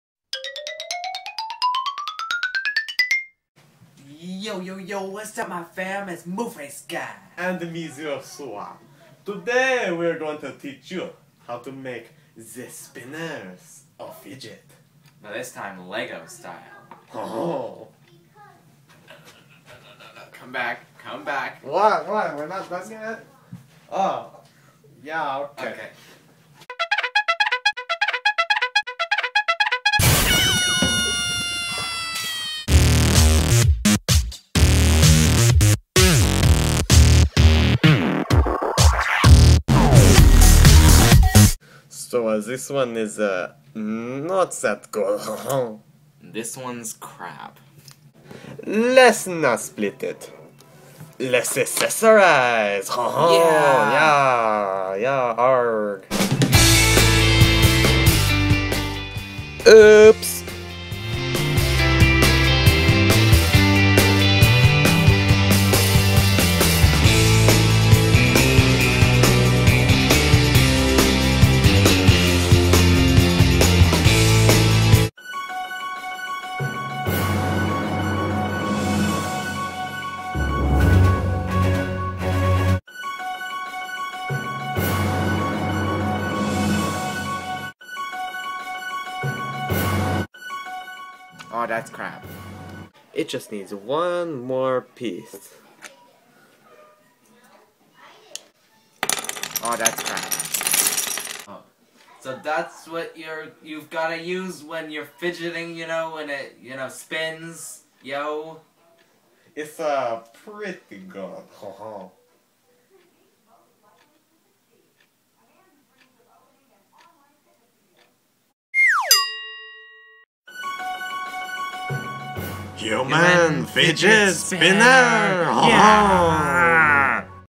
How to Lego Fidget Spinner, Yo
In this video Moofaceguy and Monsieur Soir teach you how to make a fidget spinner out of Legos. It's gonna be pretty nice.